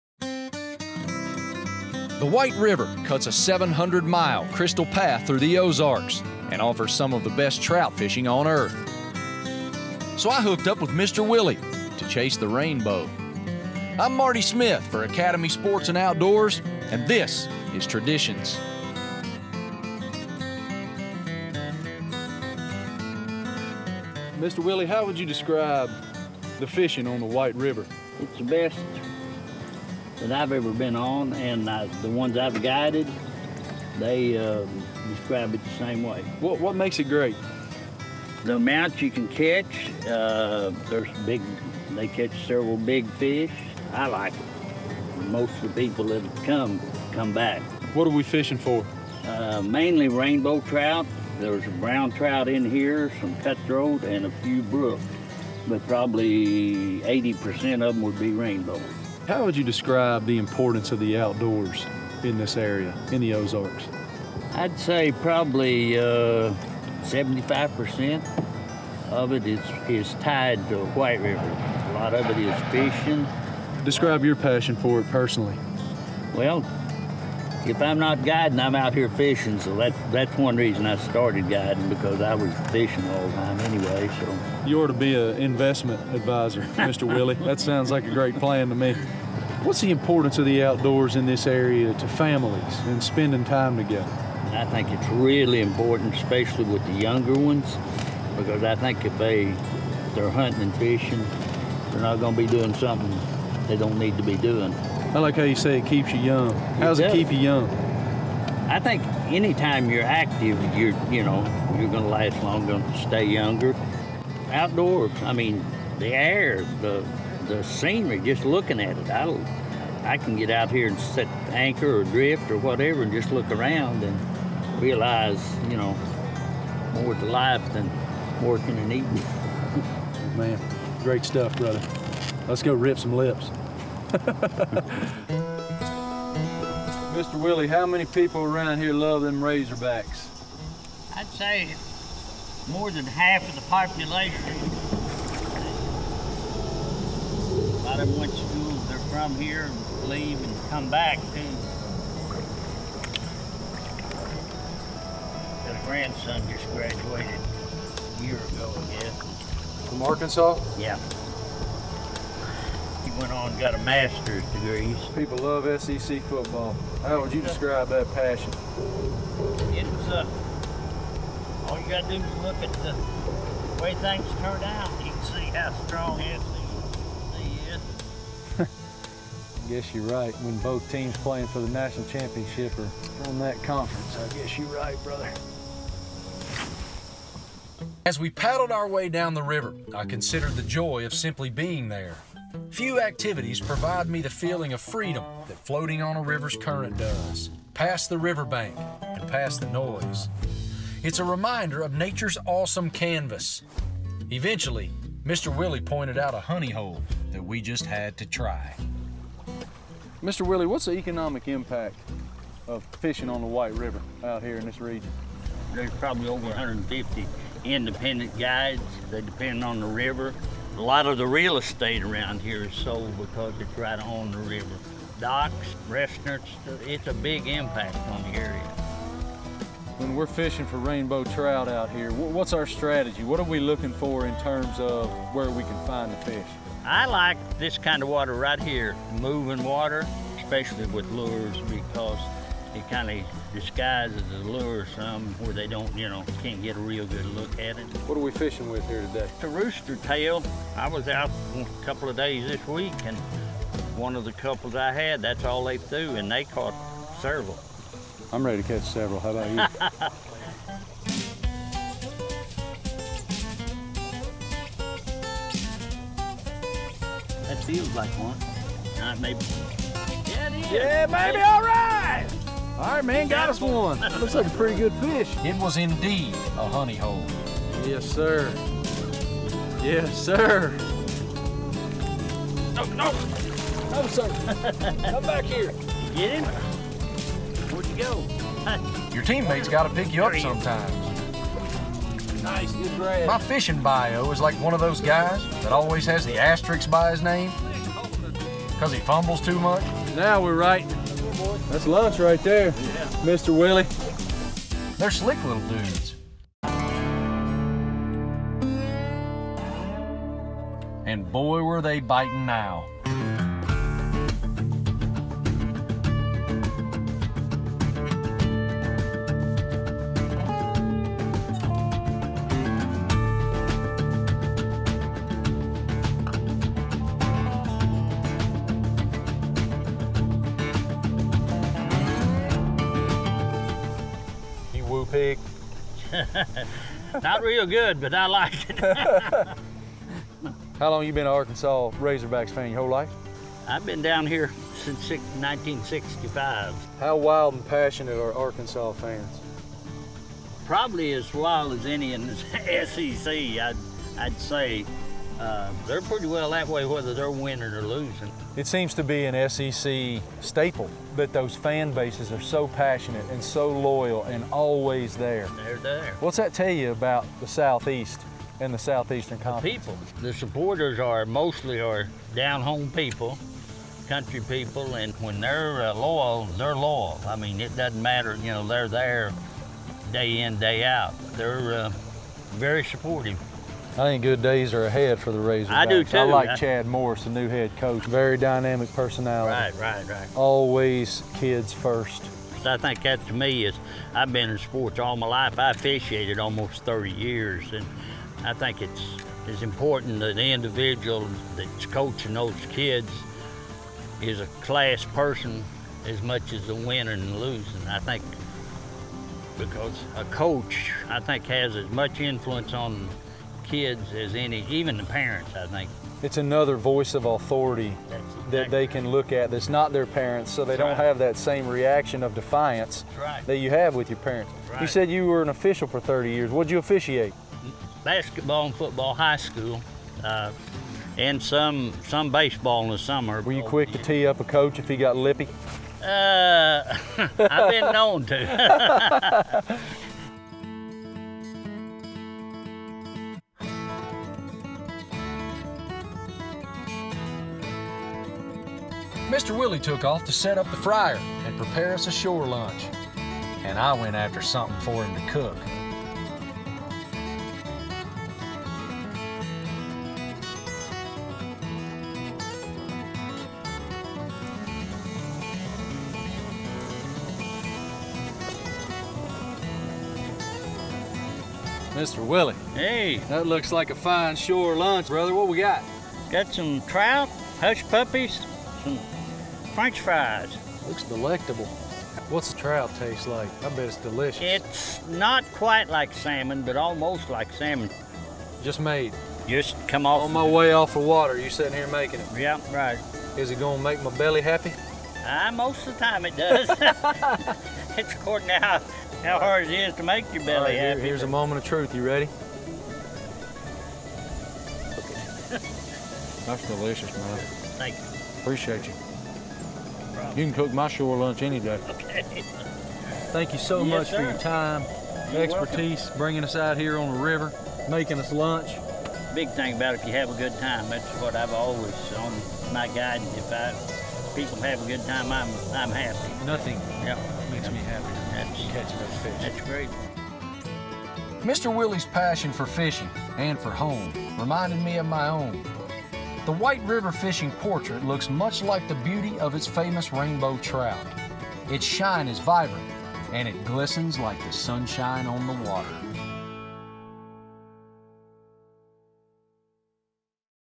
SEC Traditions | Rainbow Trout on the White River with Marty Smith
ESPN’s Marty Smith heads out on a winding stretch with a local guide on a quest for rainbow trout, while getting a lesson in the river’s impact, the freedom it provides and the area’s passion for Arkansas athletics and the SEC.

The White River winds its way through the heart of the Ozarks, providing a way of life for folks up and down its banks.